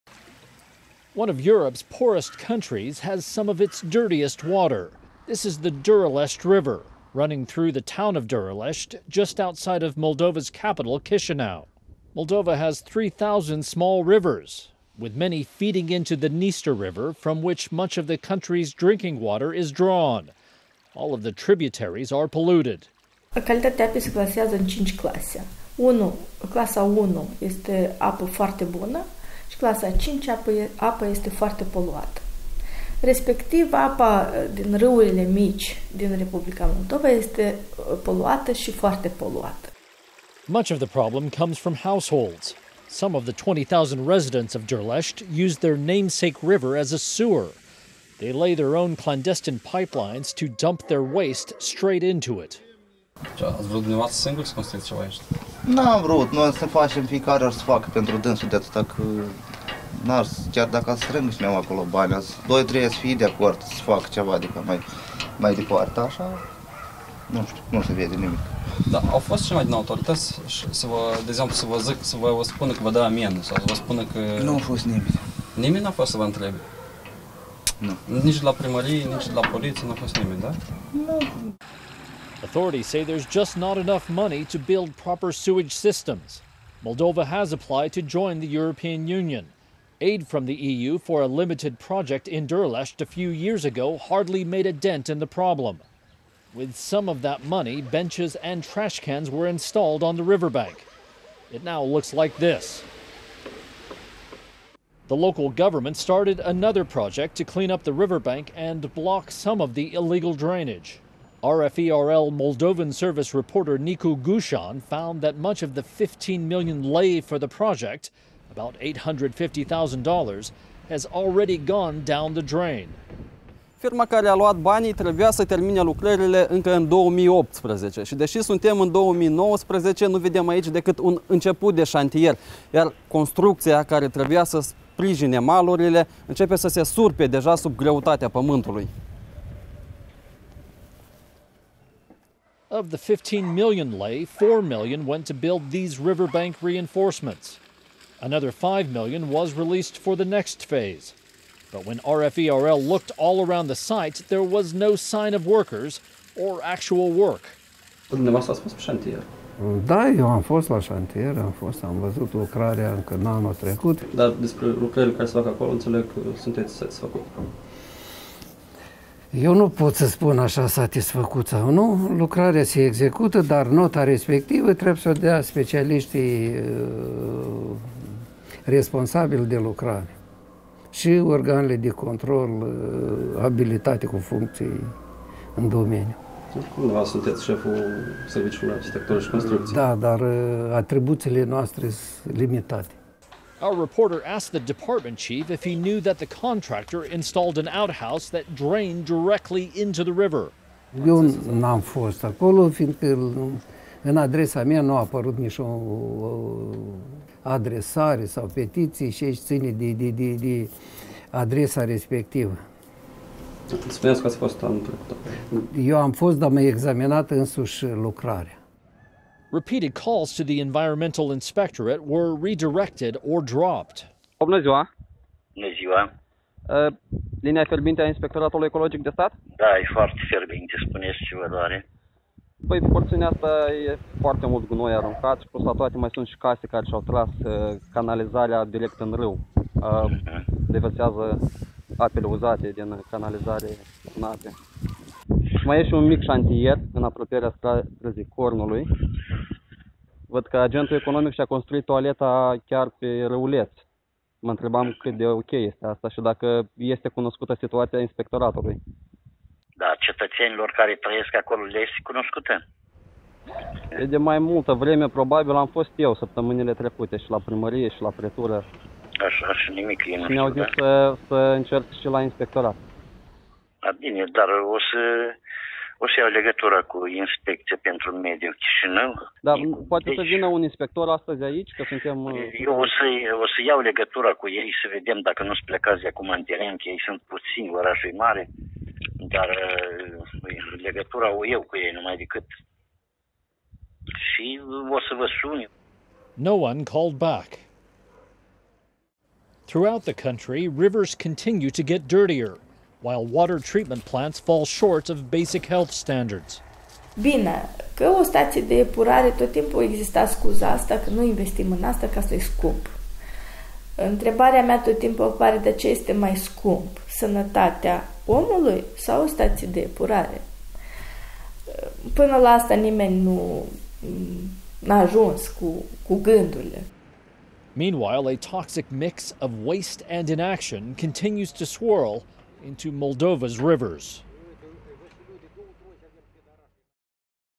Moldova's Dirty Rivers: Money And Muck Flushed Down The Drain
Moldova has some of the most polluted rivers in Europe, due in part to a lack of public sewers. The town of Durlesti shows how private solutions and a failure of public action combine in a toxic mix.